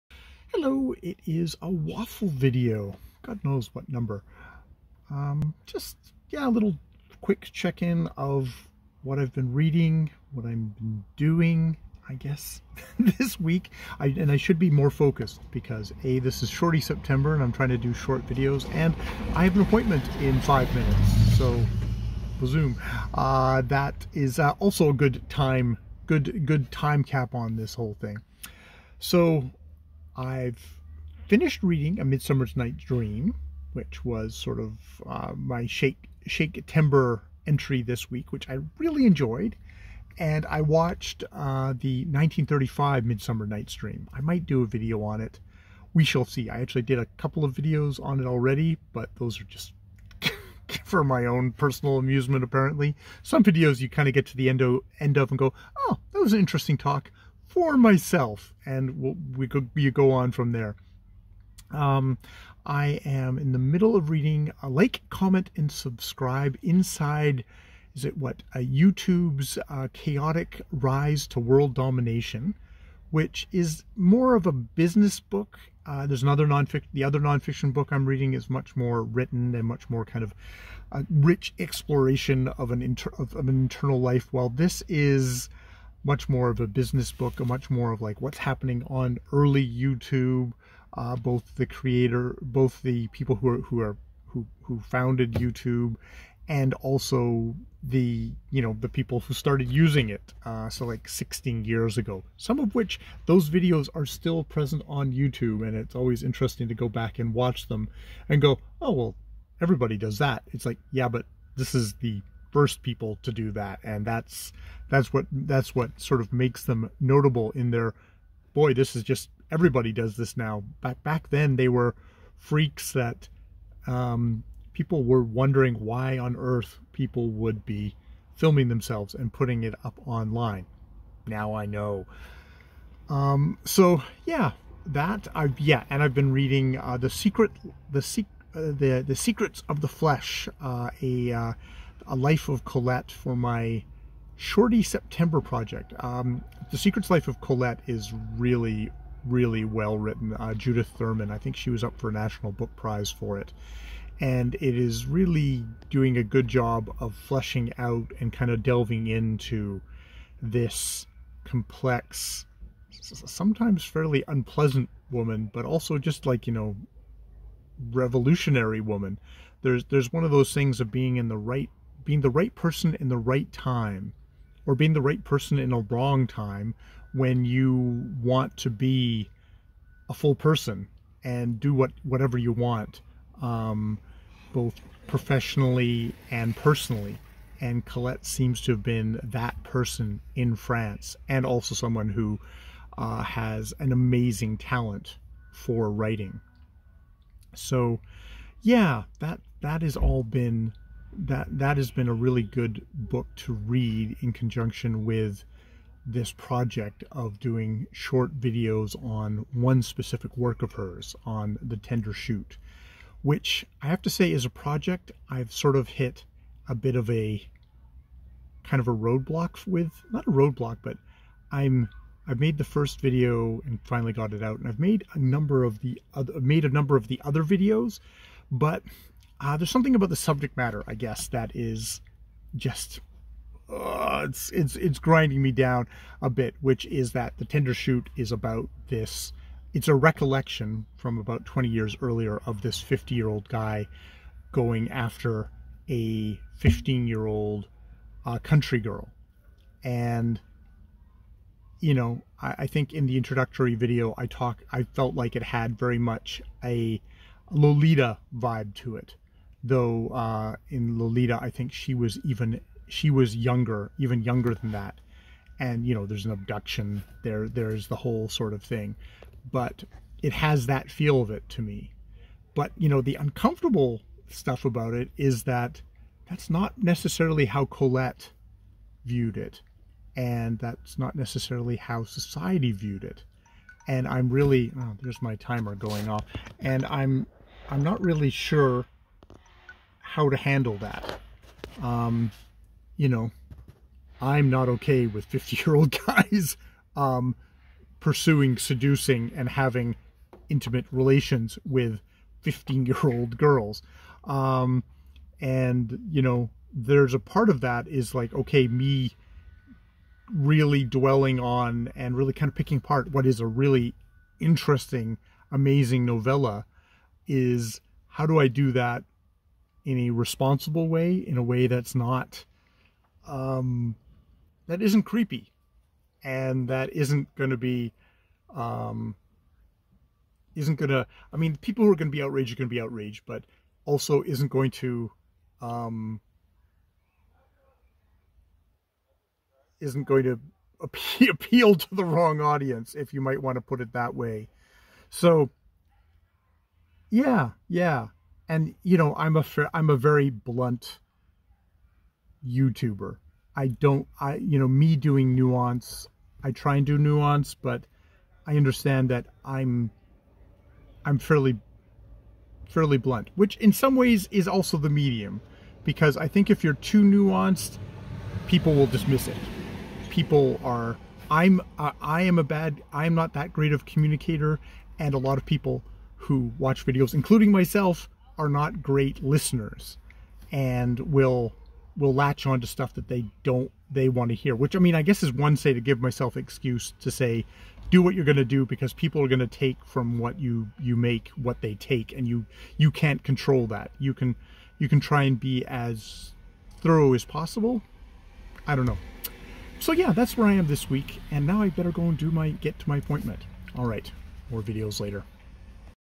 waffle # 10 (awkward)
by Mark Bergen
2022
(I forgot to identify the author in the video! Bad booktuber!!!)

Other Books and Media - mentioned properly...

A Midsummer Night's Dream (1935)
directed by Max Reinhardt and William Dieterle

‘The Tender Shoot’ from The Tender Shoot by Colette,
translated by Antonia White, 1958,
originally published in French in 1943 (according to Thurman).

Lolita
by Vladimir Nabokov
1955

Secrets of the Flesh: A Life of Colette
Judith Thurman
1999

(0013)